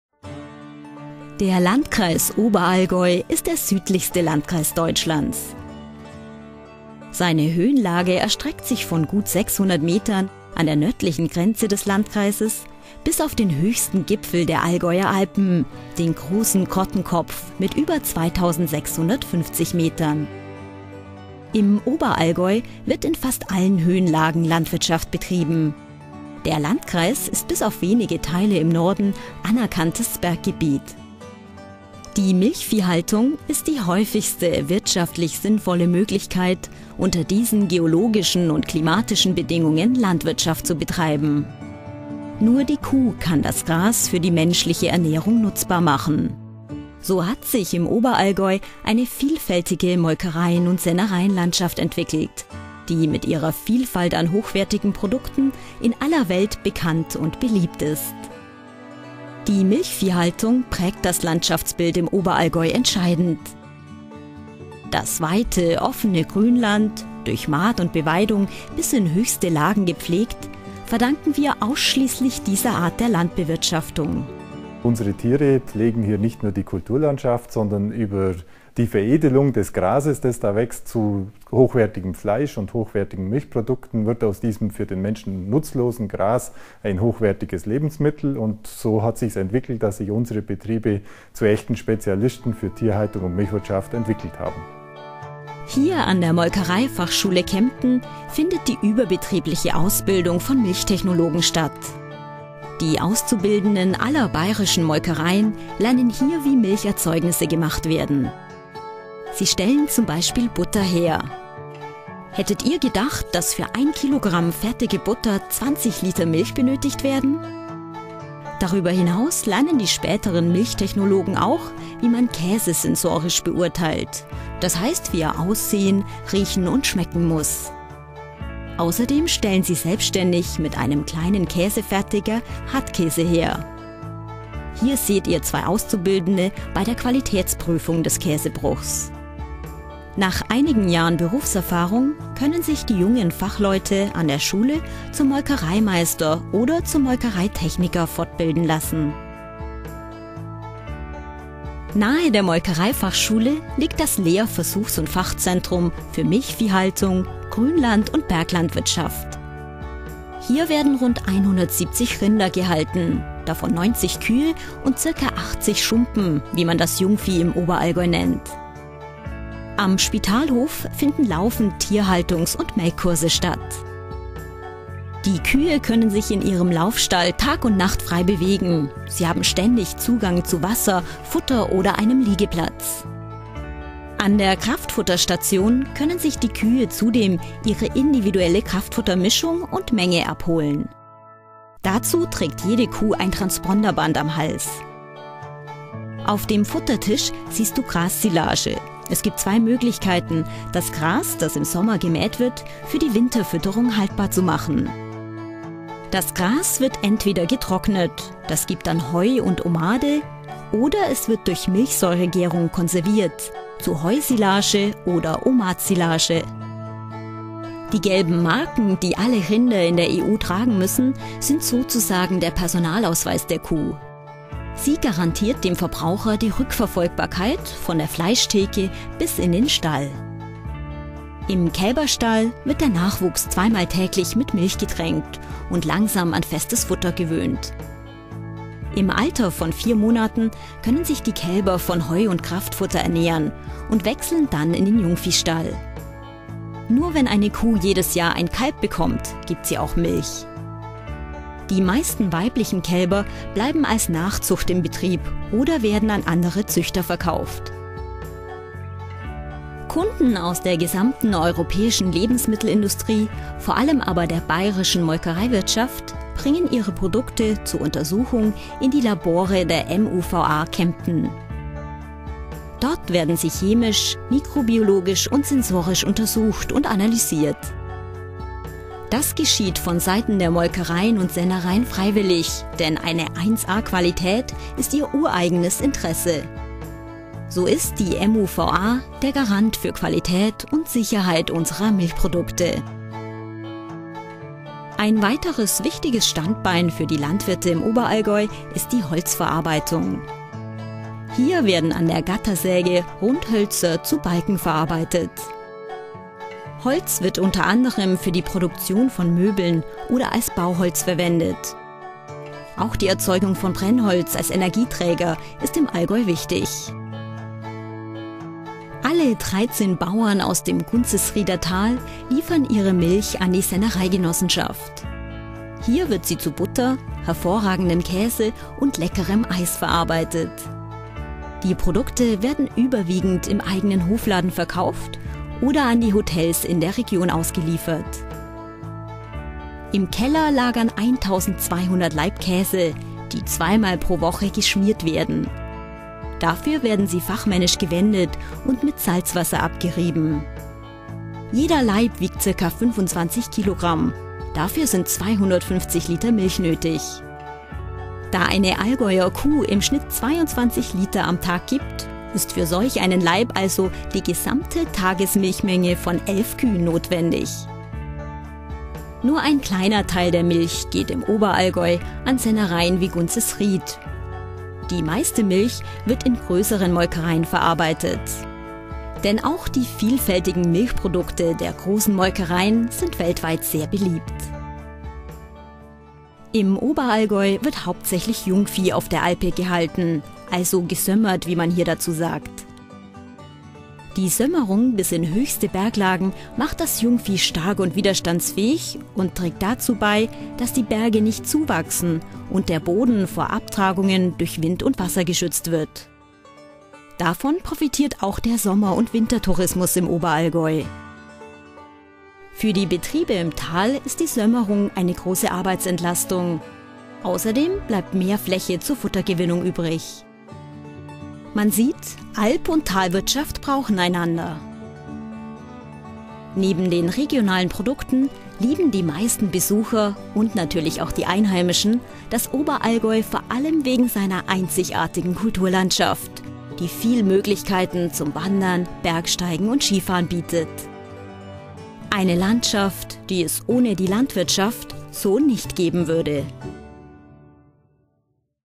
Landwirtschaft im Oberallgäu
► Landwirtschaft im Oberallgäu: Woher kommt die Milch? Wie entsteht die Butter? Was ist ein Milchtechnologe? Die Antwort auf diese und weitere spannenden Fragen erfährst Du in diesem Video über das Oberallgäu.✔

Bayerischer Bauernverband
Körperschaft des öffentlichen Rechts
Präsident Walter Heidl
Max-Joseph-Str. 9
80333 München